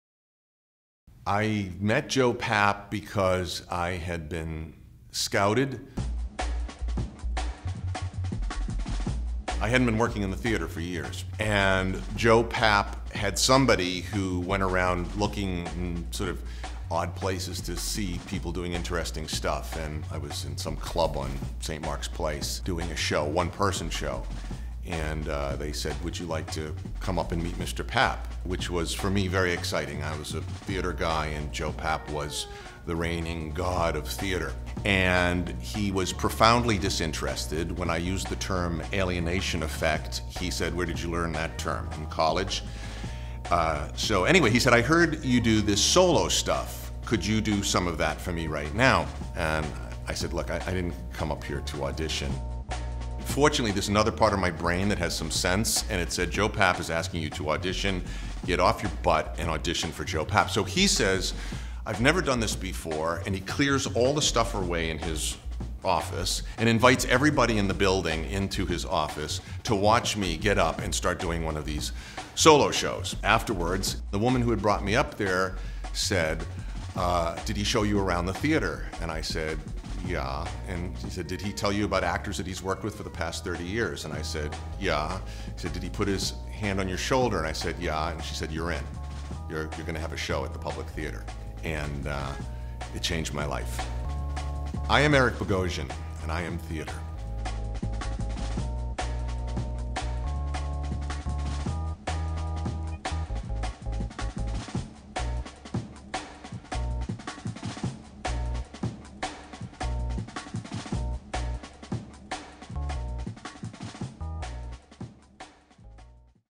I AM THEATRE: Eric Bogosian
Eric Bogosian is besk known as the author of the plays TALK RADIO and subUrbia as well as a number of Obie-award winning Off-Broadway solos, including SEX, DRUGS, ROCK & ROLL. Last season he starred on Broadway in Donald Margulies TIME STANDS STILL. Eric is the recipient of the Berlin Film Festival Silver Bear and the Drama Desk Award. He is a Guggenheim fellow and Pulitzer nominee. Eric is originally from Woburn, Massachusetts.